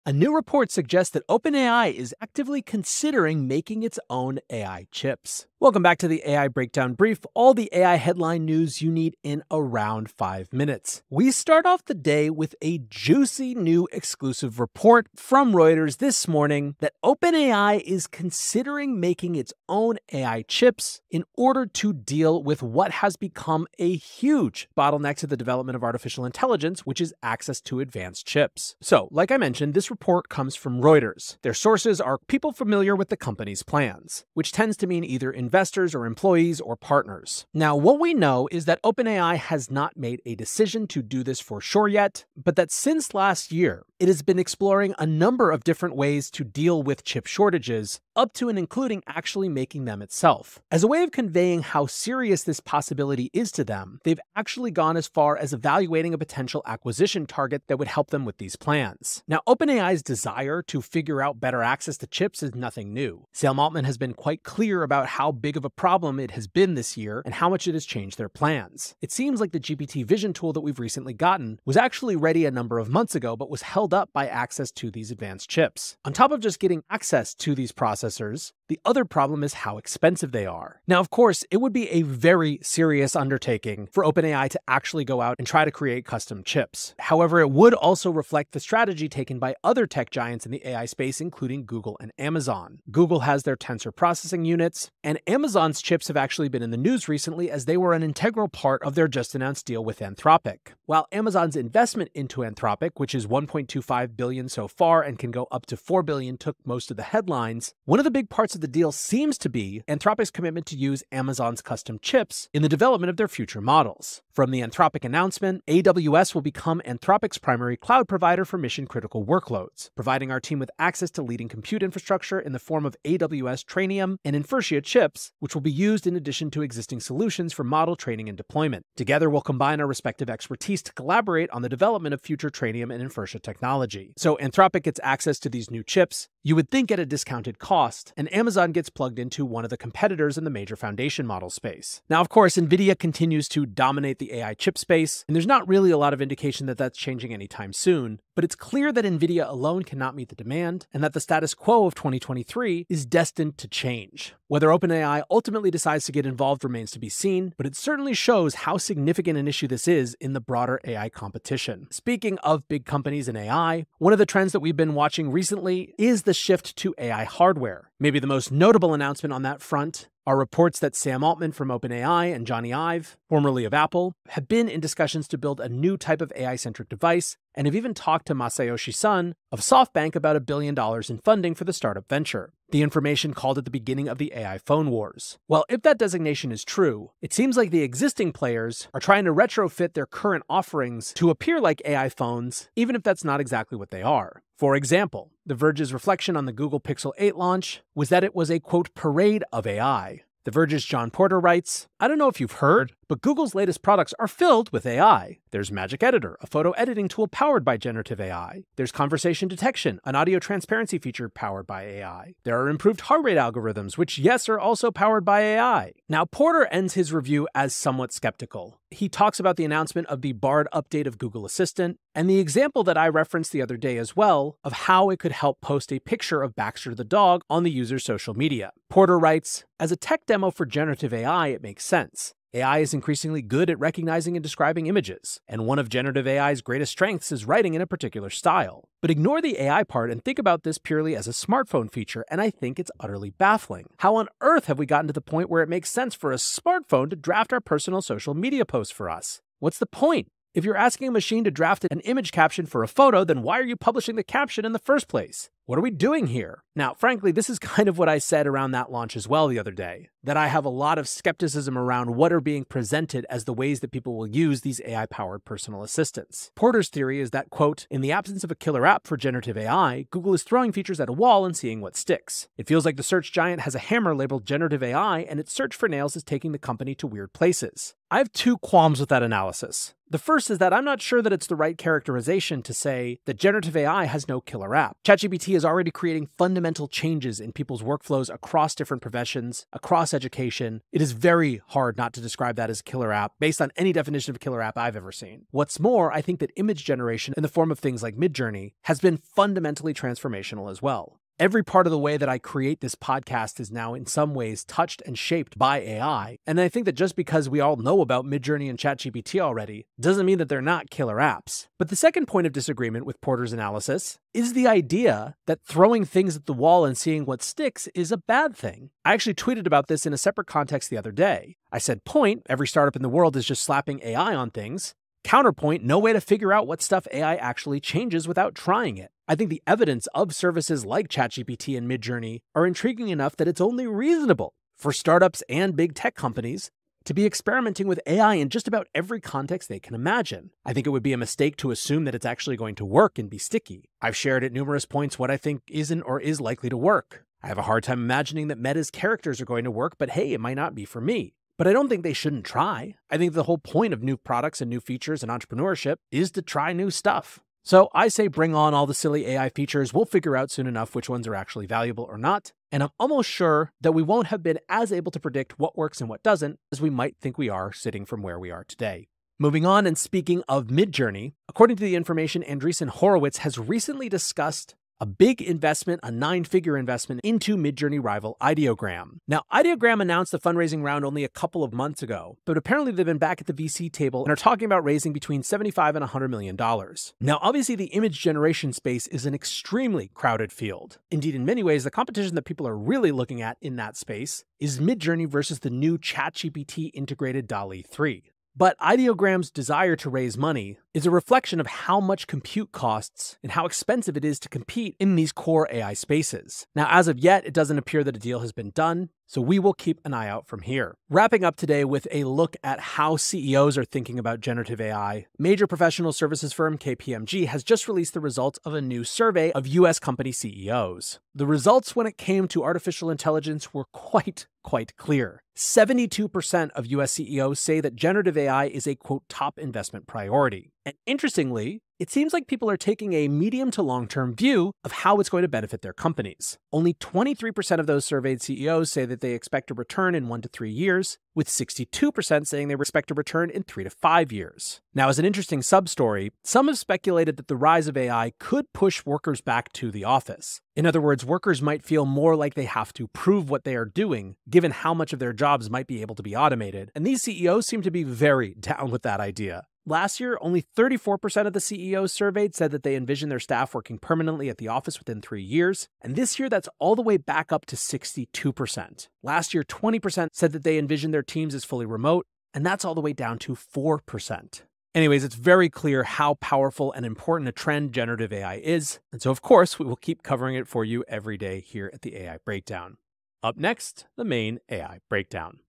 OpenAI To Make Its Own AI Chips?
Reuters reports that OpenAI has gone as far as evaluated an acquisition opportunity in order to make their own chips. Also on the Brief, what CEOs think of generative AI.